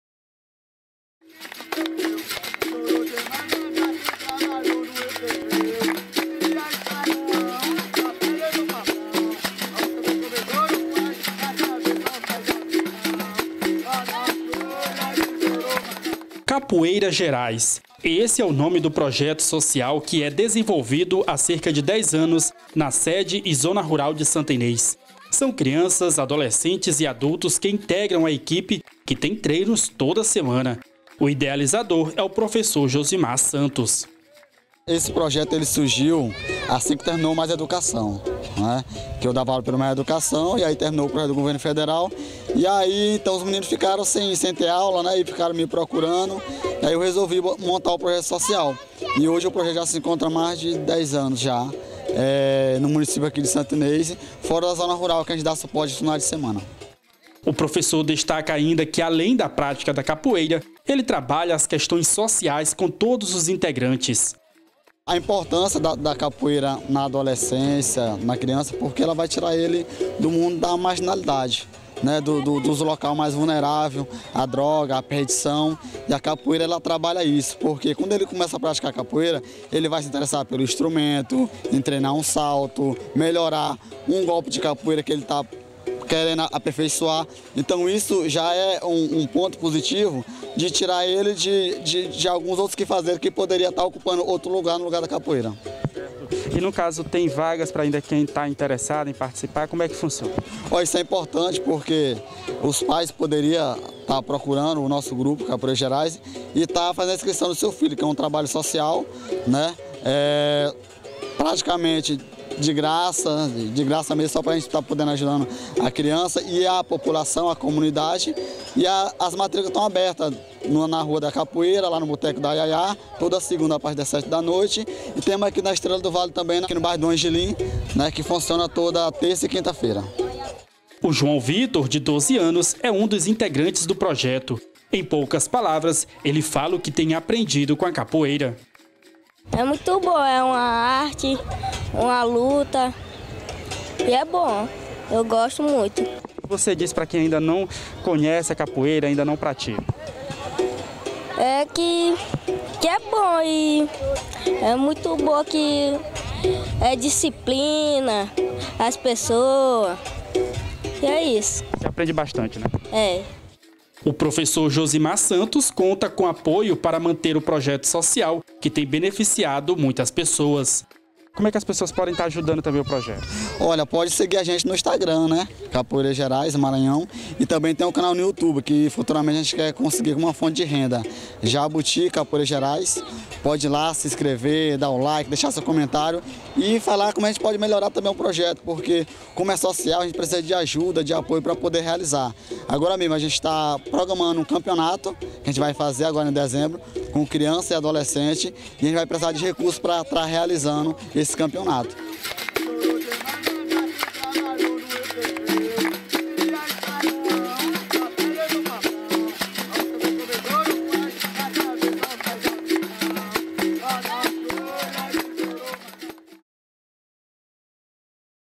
TV Remanso - CAPOEIRA E ARTE
O projeto social Capoeira Gerais está em atividade em Santa Inês há cerca de 10 anos e atende crianças e adultos. Os treinos acontecem semanalmente e ensinam a arte da capoeira, além de valores e disciplina. Saiba como você pode participar e apoiar o projeto!